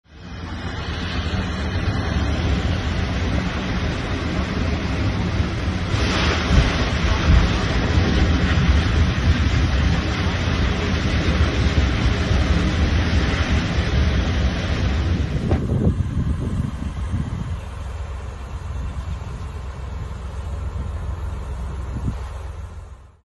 Strażacy walczą z pożarem w składzie butli z gazem na ulicy Portowej w Głogowie
reporter: Leszek Wspaniały